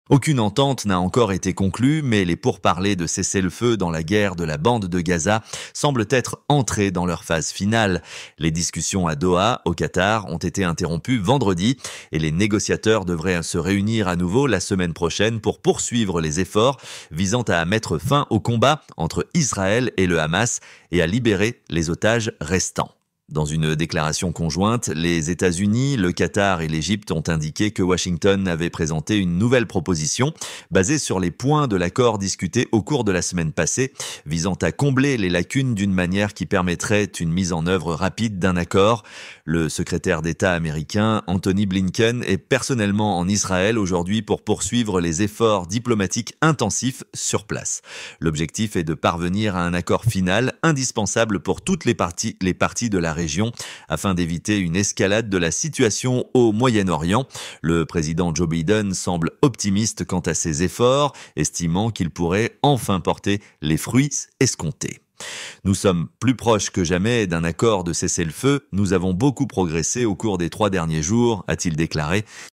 Guerre à Gaza, Biden optimiste : "Nous sommes très proches d'un cessez-le-feu !"
Aucune entente n'a encore été conclue, mais les pourparlers de cessez-le-feu dans la guerre de la bande de Gaza semblent être entrés dans leur phase finale.